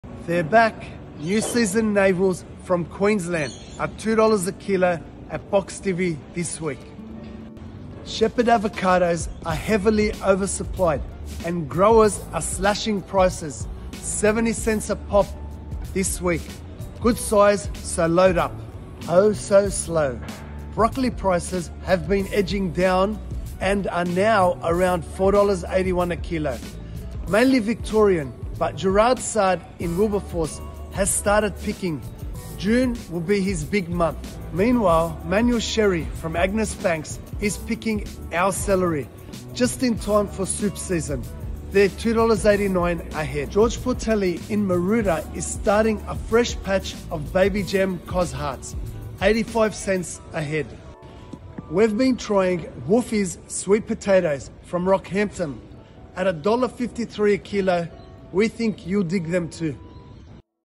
Moussa's Market Update 2nd May
A real mixed bag this week: some incredible bargains (70c avocadoes), along with some very expensive lines (capsicum around $12.50 a kilo) – but most fruit and vegetables are reasonably priced.